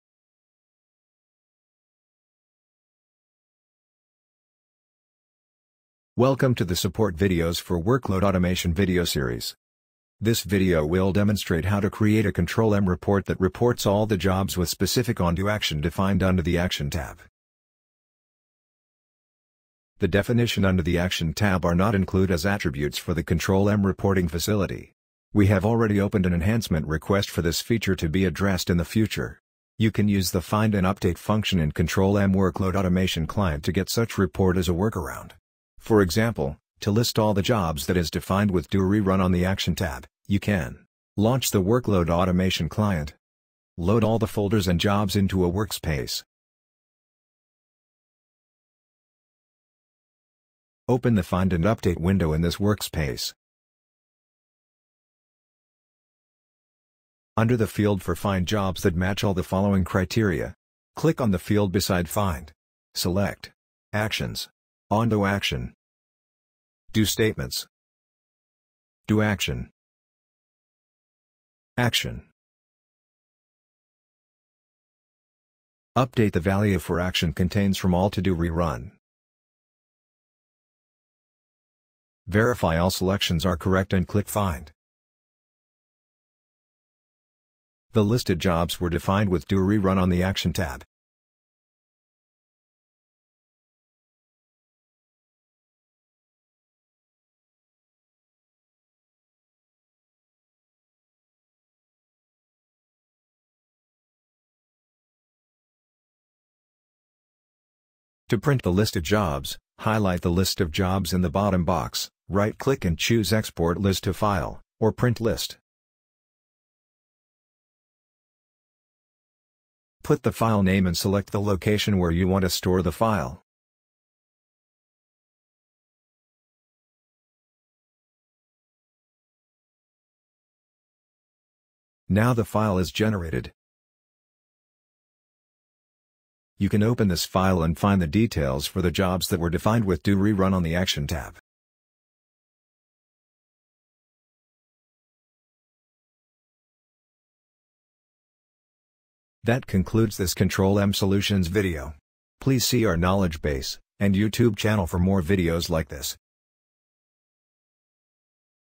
How can I create Control-M report that would show ON - DO statements under the Action Tab?
This video demonstrates how to get a list of Control-M job definition that has ONDO statements defined under Action Tab.